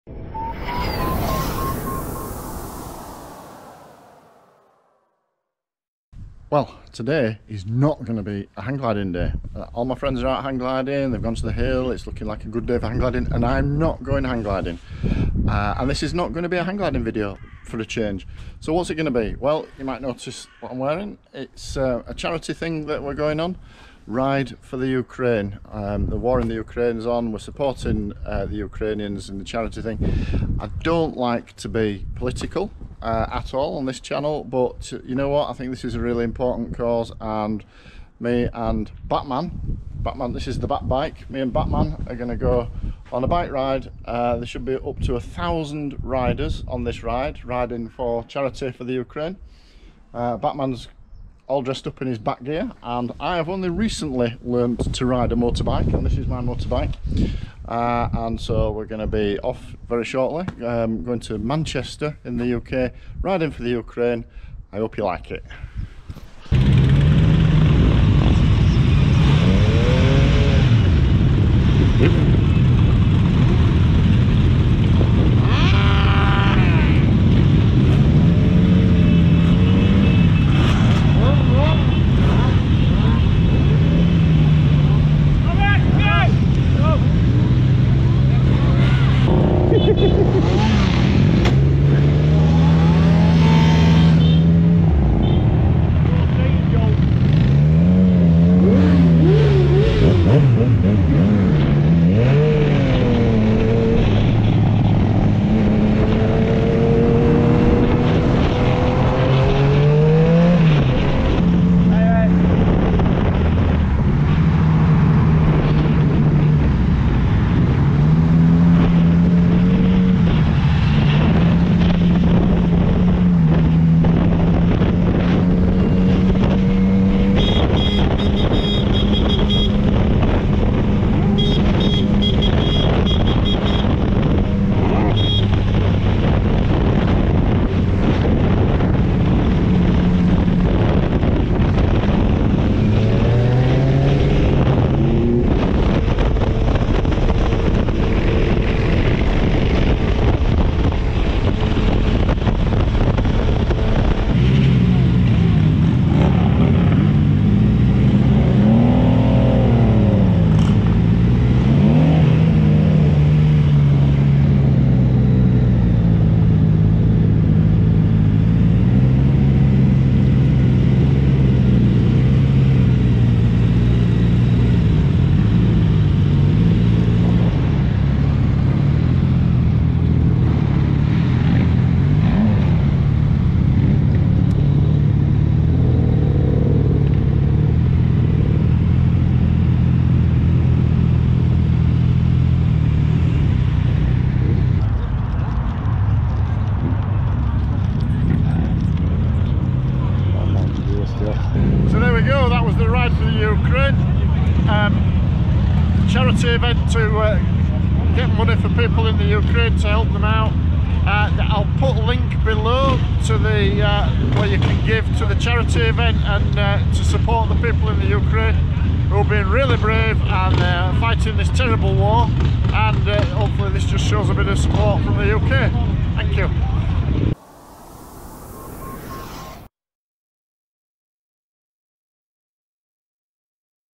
Ride for Ukraine - charity motorcycle event, Manchester, UK 20 March 2022
A short video showing clips from the Ride From Ukraine - giving our support and raising money for the Ukraine.